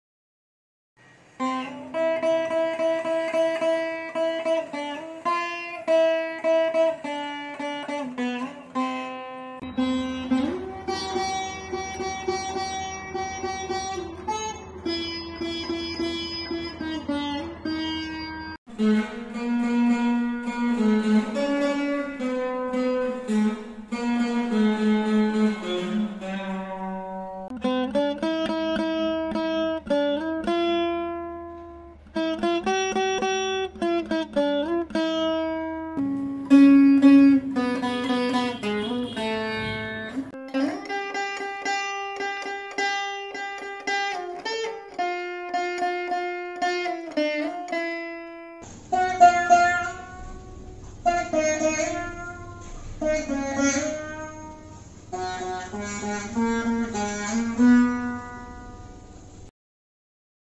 Happy Republic Day- A Sudden Unprepared Tribute by my Students.
Not much instructions given
Not much Specifications given
Not much Code given

Just as a Sudden Idea that blossomed into a simple Tribute by my Students.

Dr.Rajalakshmi